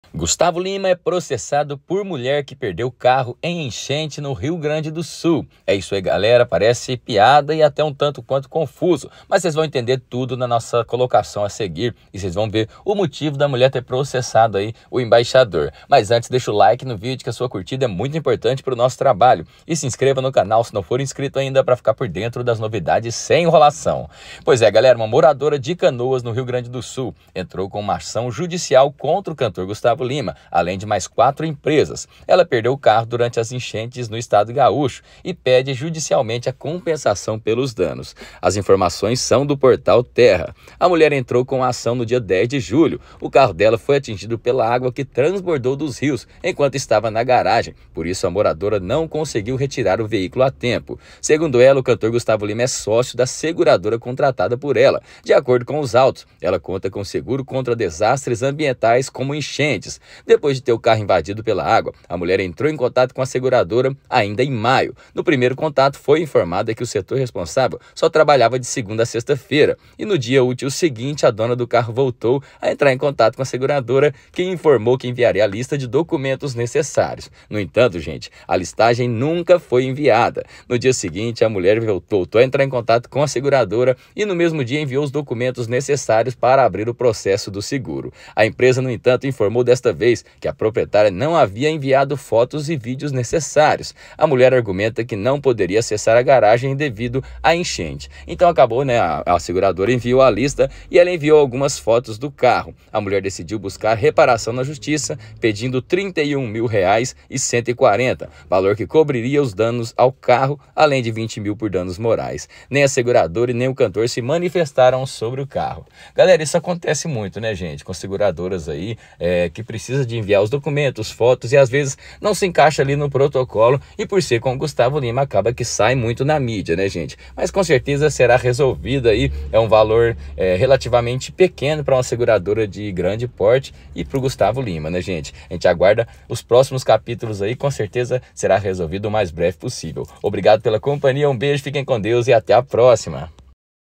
Gusttavo Lima é PROCESS4DO por MULHER que PERDEU o CARRO em ENCH3NTE no RS
Gusttavo Lima foi proc3ssado, juntamente com a empresa da qual é sócio, por quê uma mulher havia contratado os serviços da seguradora e perdeu seu carro nas enchentes do Rio Grande do Sul